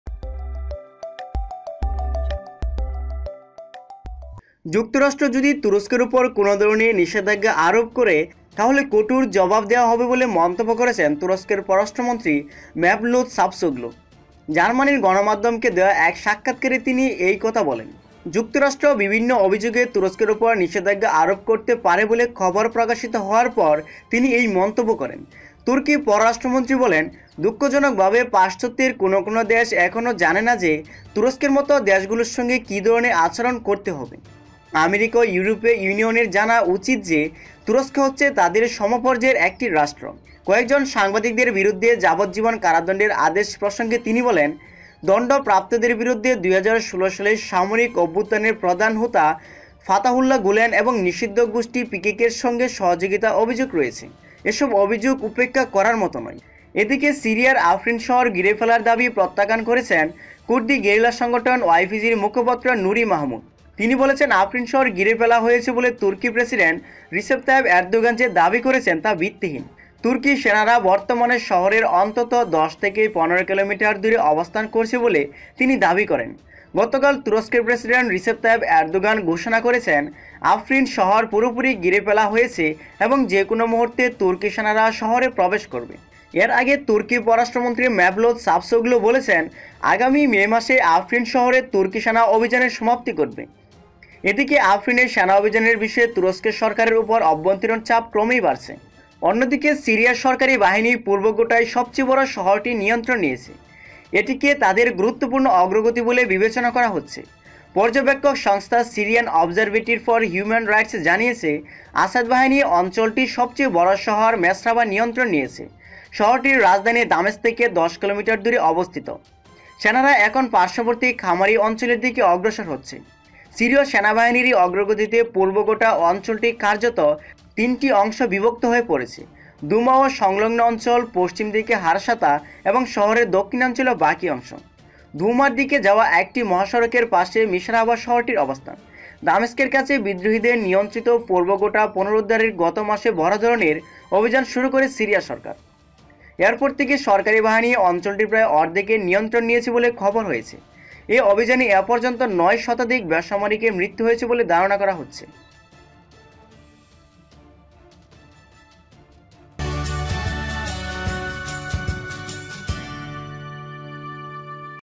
যুক্তরাষ্ট্রকে কড়া হুঁশিয়ারি তুরস্কের ! আফরিন শহর ঘেরাওয়ের দাবি এরদোগানের- ওয়াইপিজি বলল ভিত্তিহীন
যুক্তরাষ্ট্রকে কড়া হুঁশিয়ারি তুরস্কের

যুক্তরাষ্ট্র যদি তুরস্কের ওপর কোনো ধরনের নিষেধাজ্ঞা আরোপ করে তাহলে কঠোর জবাব দেয়া হবে বলে মন্তব্য করেছেন তুরস্কের পররাষ্ট্রমন্ত্রী মেভলুত চাভুসওগ্লু। জার্মানির গণমাধ্যমকে দেয়া এক সাক্ষাৎকারে তিনি এ কথা বলেন।

যুক্তরাষ্ট্র বিভিন্ন অভিযোগে তুরস্কের ওপর নিষেধাজ্ঞা আরোপ করতে পারে বলে খবর প্রকাশিত হওয়ার পর তিনি এ মন্তব্য করেন।

তুর্কি পররাষ্ট্রমন্ত্রী বলেন, দুঃখজনকভাবে পাশ্চাত্যের কোনো কোনো দেশ এখনও জানে না যে, তুরস্কের মতো দেশগুলোর সঙ্গে কী ধরনের আচরণ করতে হবে। আমেরিকা ও ইউরোপীয় ইউনিয়নের জানা উচিত যে, তুরস্ক হচ্ছে তাদের সমপর্যায়ের একটি রাষ্ট্র।

 আফরিন শহর ঘেরাওয়ের দাবি এরদোগানের- ওয়াইপিজি বলল ভিত্তিহীন

সিরিয়ার আফরিন শহর ঘিরে ফেলার দাবি প্রত্যাখ্যান করেছেন কুর্দি গেরিলা সংগঠন ওয়াইপিজি'র মুখপাত্র নুরি মাহমুদ। তিনি বলেছেন, আফরিন শহর ঘিরে ফেলা হয়েছে বলে তুর্কি প্রেসিডেন্ট রজব তাইয়্যেব এর্দোগান যে দাবি করেছেন তা ভিত্তিহীন।

তুর্কি সেনারা বর্তমানে শহরের অন্তত ১০ থেকে ১৫ কিলোমিটার দূরে অবস্থান করছে বলে তিনি দাবি করেন। গতকাল (শুক্রবার) তুরস্কের প্রেসিডেন্ট রজব তাইয়্যেব এর্দোগান ঘোষণা করেছেন, আফরিন শহর পুরোপুরি ঘিরে ফেলা হয়েছে এবং যেকোনো মুহূর্তে তুর্কি সেনারা শহরে প্রবেশ করবে।

এর আগে তুর্কি পররাষ্ট্রমন্ত্রী মভলুদ চাভেজওগ্লু বলেছেন, আগামী মে মাসেই আফরিন শহরে তুর্কি সেনা অভিযানের সমাপ্তি ঘটবে।

এদিকে, আফরিনে সেনা অভিযানের বিষয়ে তুরস্ক সরকারের ওপর আভ্যন্তরীণ চাপ ক্রমেই বাড়ছে। গত বুধবার দেশটির সংসদে এই ইস্যুতে সরকার ও বিরোধী দলের সংসদ সদস্যদের মধ্য হাতাহাতি হয়েছে। এর ফলে বিরোধী দলের দুইজন এবং ক্ষমতাসীন দলের একজন হাসপাতালে ভর্তি হয়েছে।

sirya issue and erdogan
turosko and america
bangla news
bd update news
bangla news today
bd online news today
bd news
world news